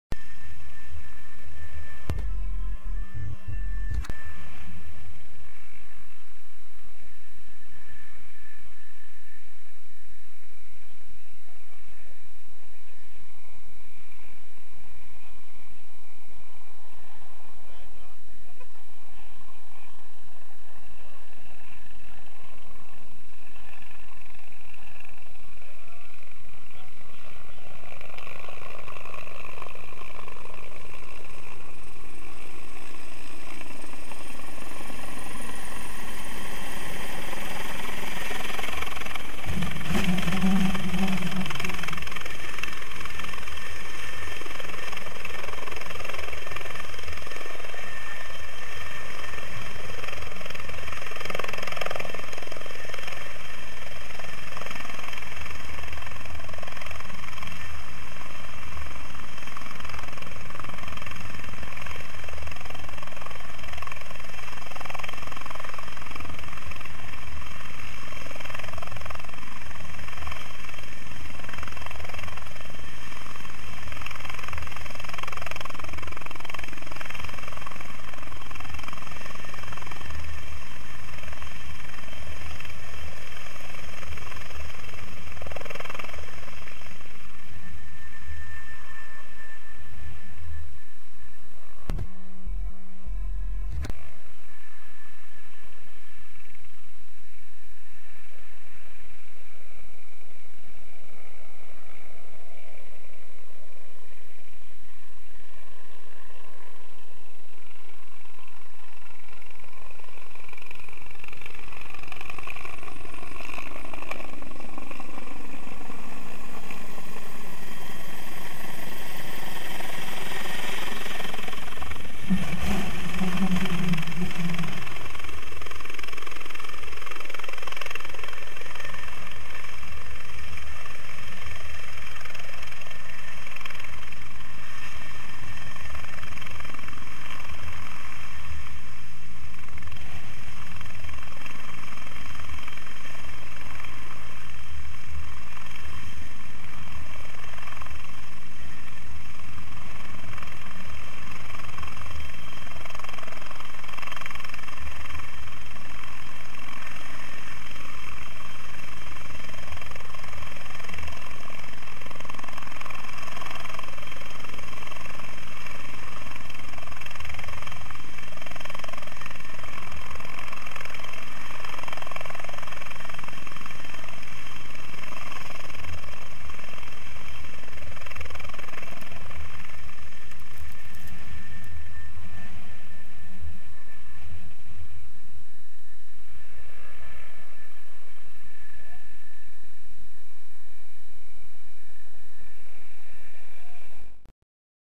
Výcvik s leteckou hasičskou službou, Krásné, 25. 04. 2015 - členové JSDH Bratrušov
Plnění Bambi vaku členy JSDH Bratrušov